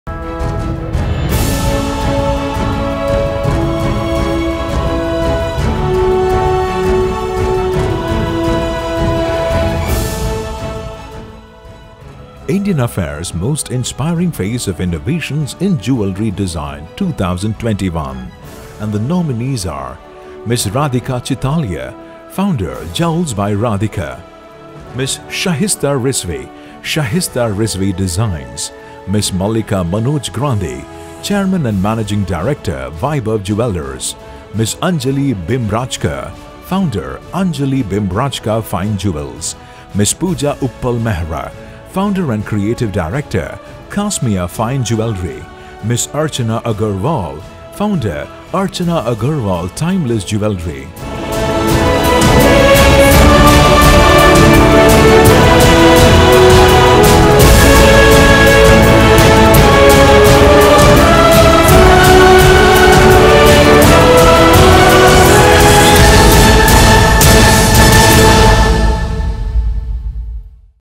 SHAHISTA RIZVI is Indian Affairs Most Inspiring Face of Innovations in Jewellery Design 2021
SHAHISTA RIZVI is Indian Affairs Most Inspiring Face of Innovations in Jewellery  Design  2021 at the iconic & prestigious 12th Annual India Leadership Conclave & Indian Affairs Business Leadership Awards 2021.